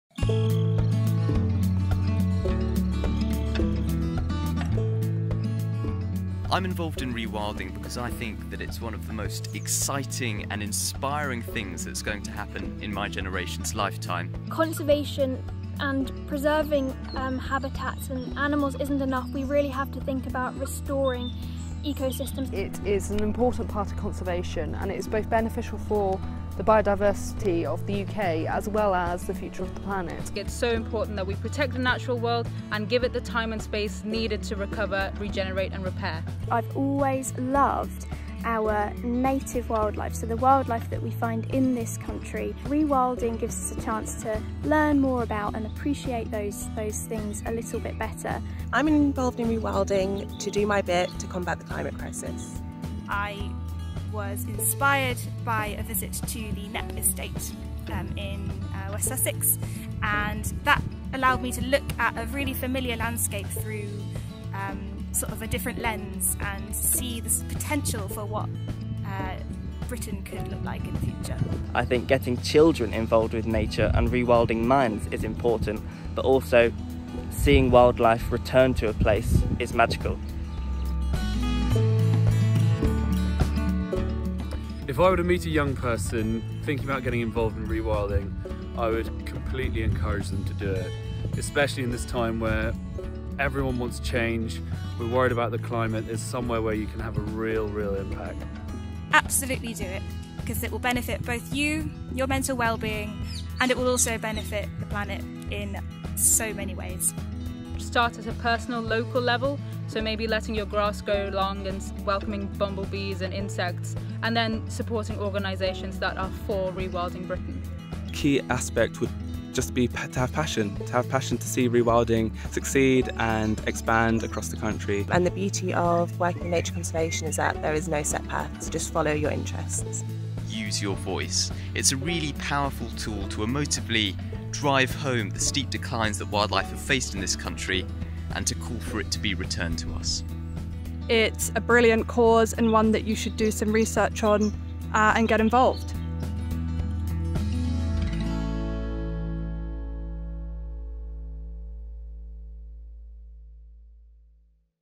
Rewilding – Why Should You Care?
Young people care about rewilding because they see it as a chance to have a
real impact for creating a future solves climate change and the biodiversity crisis.

Interviewees: James Miller, Bella Lack, Charlotte O'Neil, Sathvika Krishnan, Lucy Houliston, Chantelle Lindsay, Izzy Taylor, Andrew Millham, Ivan de Klee, Nathan Van Cooten, Mir Singhvi